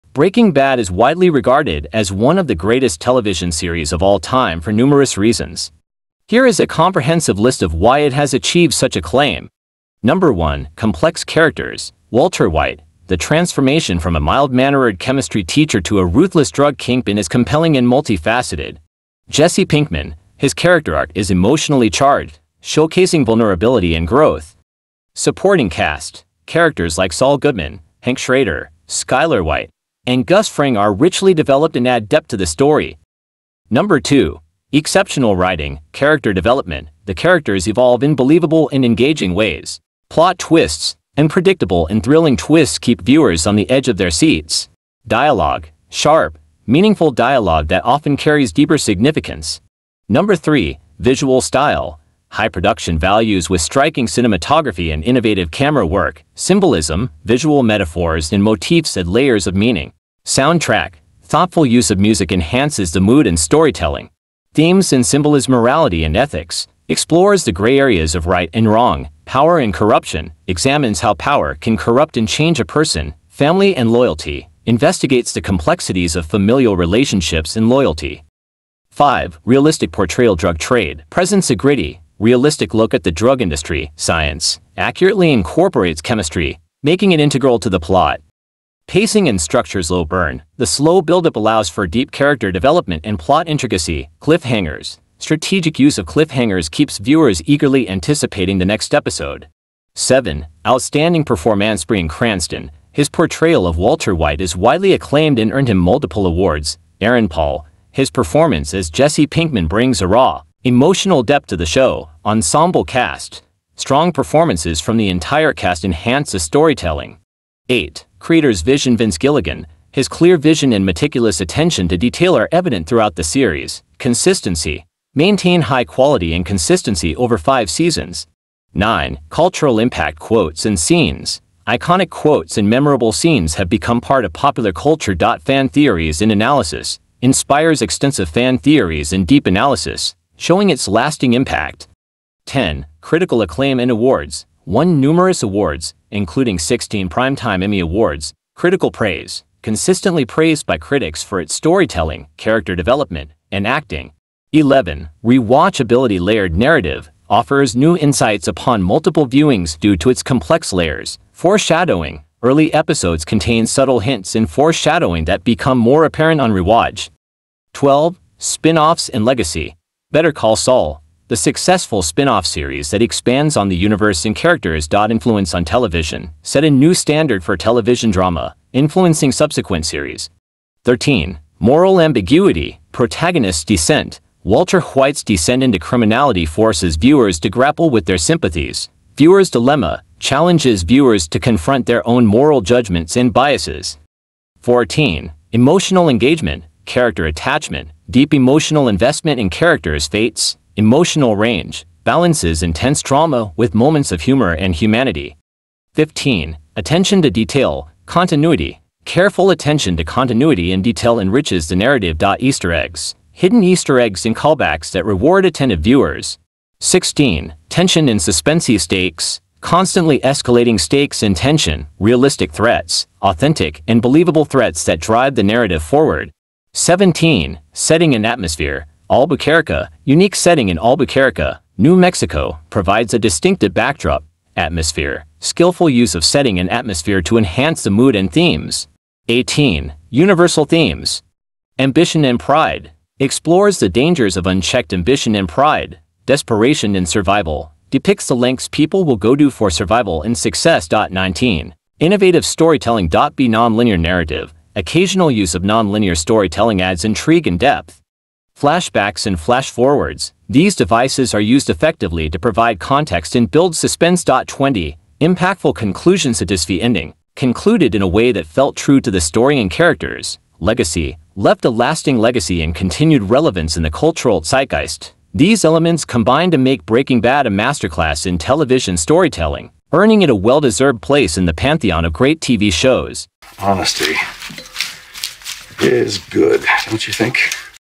Breaking Bad - Show Review
Flashbacks and Flash-forwards: These devices are used effectively to provide context and build suspense.20. Impactful ConclusionSatisfying Ending: Concluded in a way that felt true to the story and characters.Legacy: Left a lasting legacy and continued relevance in the cultural zeitgeist.These elements combine to make Breaking Bad a masterclass in television storytelling, earning it a well-deserved place in the pantheon of great TV shows.18. Universal ThemesAmbition and Pride: Explores the dangers of unchecked ambition and pride.Desperation and Survival: Depicts the lengths people will go to for survival and success.19. Innovative StorytellingNonlinear Narrative: Occasional use of nonlinear storytelling adds intrigue and depth.17. Setting and AtmosphereAlbuquerque: Unique setting in Albuquerque, New Mexico, provides a distinctive backdrop.Atmosphere: Skillful use of setting and atmosphere to enhance the mood and themes.16. Tension and SuspenseHigh Stakes: Constantly escalating stakes and tension.Realistic Threats: Authentic and believable threats that drive the narrative forward.14. Emotional EngagementCharacter Attachment: Deep emotional investment in characters’ fates.Emotional Range: Balances intense drama with moments of humor and humanity.15. Attention to DetailContinuity: Careful attention to continuity and detail enriches the narrative.Easter Eggs: Hidden Easter eggs and callbacks that reward attentive viewers.13. Moral AmbiguityProtagonist's Descent: Walter White’s descent into criminality forces viewers to grapple with their sympathies.Viewer’s Dilemma: Challenges viewers to confront their own moral judgments and biases.12. Spinoffs and LegacyBetter Call Saul: The successful spinoff series that expands on the universe and characters.Influence on Television: Set a new standard for television drama, influencing subsequent series.11. RewatchabilityLayered Narrative: Offers new insights upon multiple viewings due to its complex layers.Foreshadowing: Early episodes contain subtle hints and foreshadowing that become more apparent on rewatch.10. Critical Acclaim and AwardsAwards: Won numerous awards, including 16 Primetime Emmy Awards.Critical Praise: Consistently praised by critics for its storytelling, character development, and acting.9. Cultural ImpactQuotes and Scenes: Iconic quotes and memorable scenes have become part of popular culture.Fan Theories and Analysis: Inspires extensive fan theories and deep analysis, showing its lasting impact.8. Creator’s VisionVince Gilligan: His clear vision and meticulous attention to detail are evident throughout the series.Consistency: Maintained high quality and consistency over five seasons.7. Outstanding PerformancesBryan Cranston: His portrayal of Walter White is widely acclaimed and earned him multiple awards.Aaron Paul: His performance as Jesse Pinkman brings a raw, emotional depth to the show.Ensemble Cast: Strong performances from the entire cast enhance the storytelling.. Pacing and StructureSlow Burn: The slow build-up allows for deep character development and plot intricacy.Cliffhangers: Strategic use of cliffhangers keeps viewers eagerly anticipating the next episode.5. Realistic PortrayalDrug Trade: Presents a gritty, realistic look at the drug industry.Science: Accurately incorporates chemistry, making it integral to the plot.. Themes and SymbolismMorality and Ethics: Explores the gray areas of right and wrong.Power and Corruption: Examines how power can corrupt and change a person.Family and Loyalty: Investigates the complexities of familial relationships and loyalty.Soundtrack: Thoughtful use of music enhances the mood and storytelling.3. Cinematic QualityVisual Style: High production values with striking cinematography and innovative camera work.Symbolism: Visual metaphors and motifs add layers of meaning.Dialogue: Sharp, meaningful dialogue that often carries deeper significance.2. Exceptional WritingCharacter Development: The characters evolve in believable and engaging ways.Plot Twists: Unpredictable and thrilling twists keep viewers on the edge of their seats.